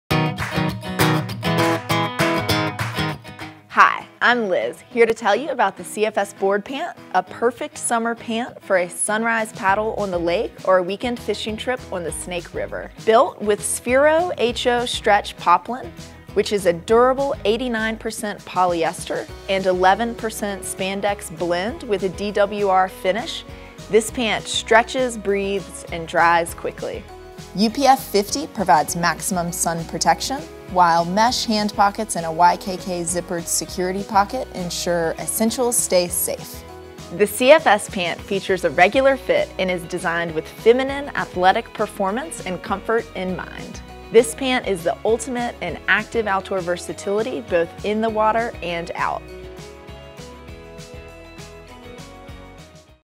Women's CFS Board Pant
Sunrise SUP on the lake, work, PTA meeting—it takes a special woman to keep up with mountain life, and it takes a special pair of pants to keep up with her. Our CFS Pant walks the walk. Created with Sphero™ HO stretch poplin, this pant adaptably wicks moisture, stretches, breathes, and dries quickly. Designed with sleek, fitted lines, the CFS Pant is intended to get you from dawn to dusk comfortably, and UPF 50+ provides excellent protection from the sun. A modern straight-leg style and gusseted crotch for ease of movement make these pants as at home on any rock face as they are in the office.